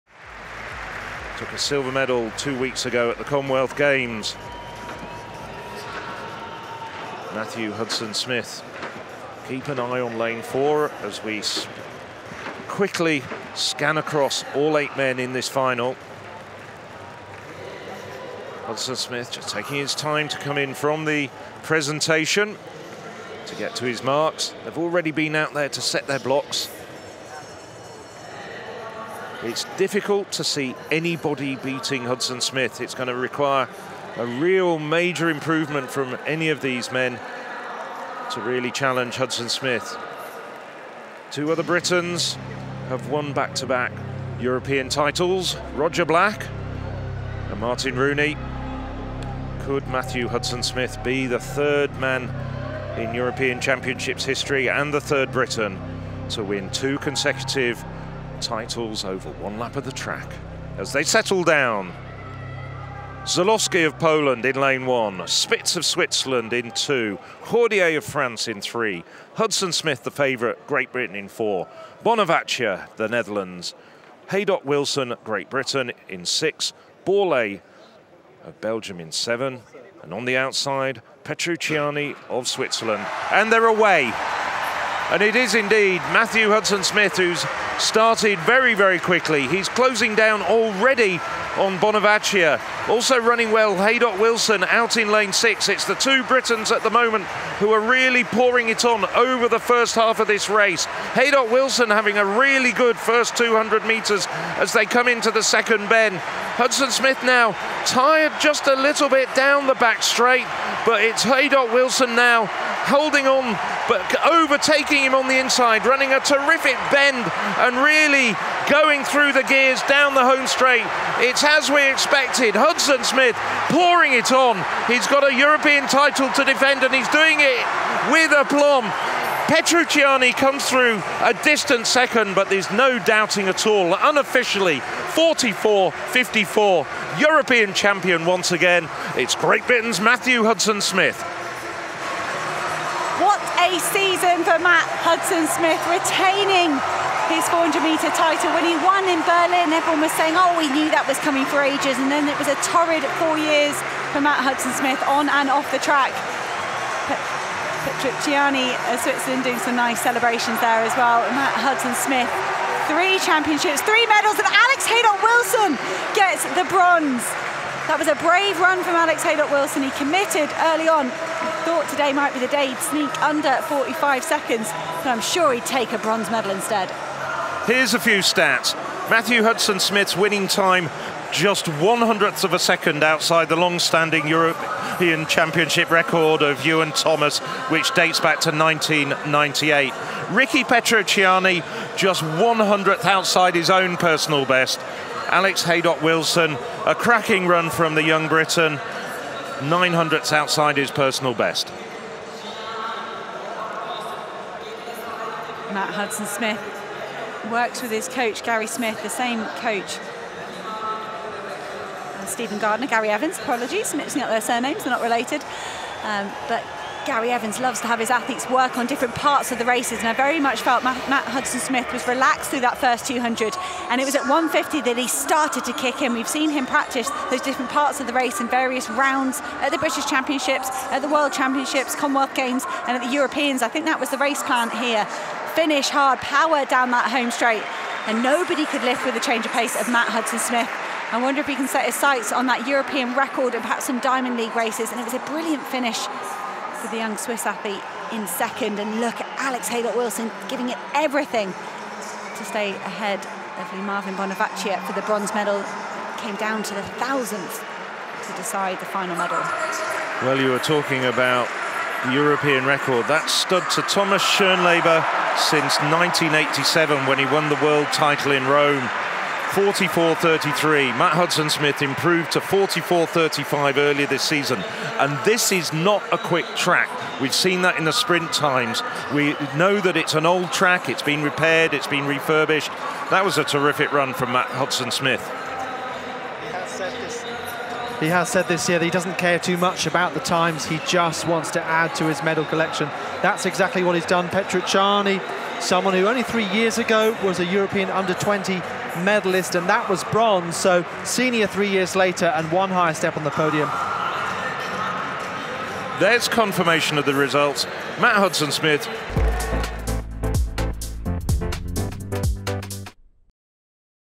Men's 400m Final | Munich 2022 | Matthew Hudson-Smith
Great Britain & Northern Ireland's Matthew Hudson-Smith successfully defended his 400m title at the European Athletics Championships.

Hudson-Smith comfortably claimed gold in Munich, clocking 44.53 to add to his Commonwealth Games bronze medal he won this year.

Switzerland's Ricky Petrucciani and fellow Brit Alex Haydock-Wilson earned silver and bronze respectively.

Watch the full race replay now!